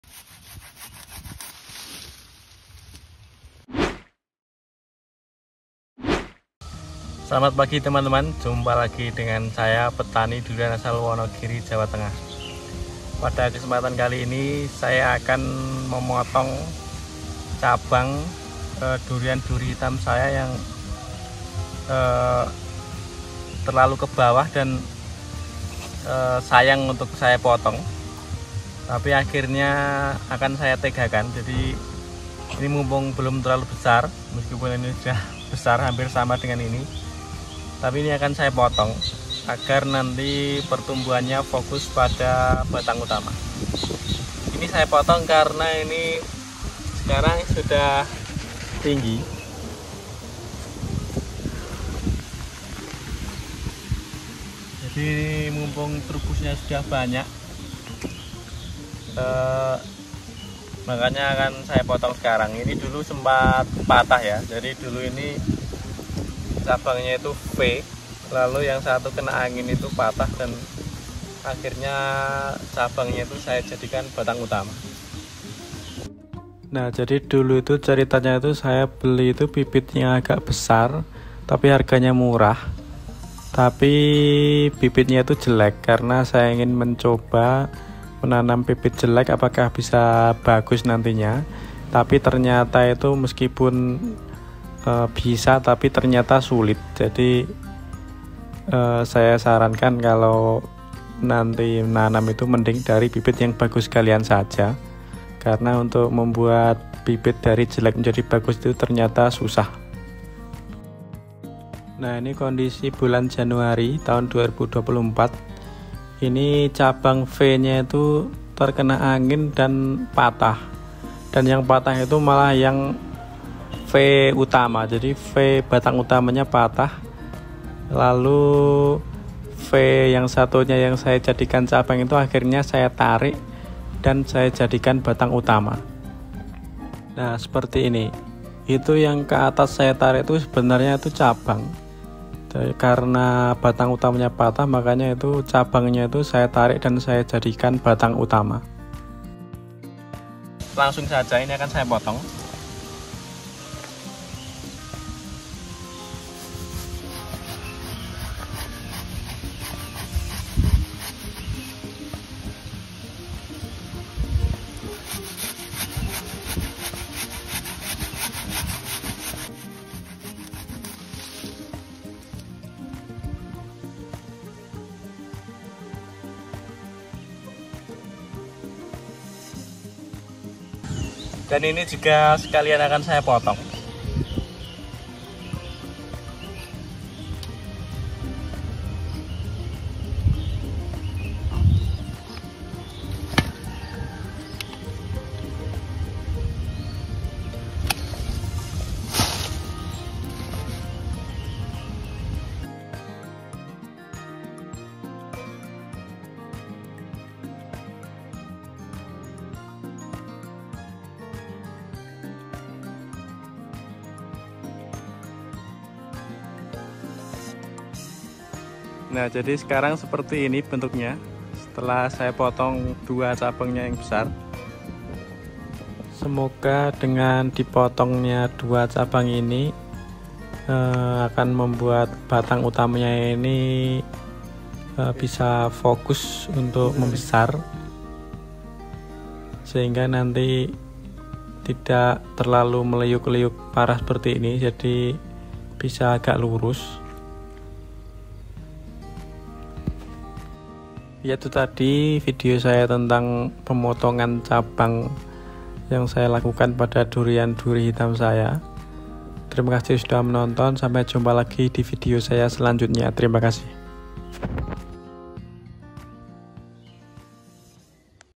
PEMOTONGAN CABANG BESAR DURIAN OCHEE/BLACKTHORN. BIAR APA???
Kali ini saya akan memotong cabang durian duri hitam saya yang kena masalah terus sejak tanam. Dulu sempat patah kena angin. Seperti apa hasilnya? Simak videonya.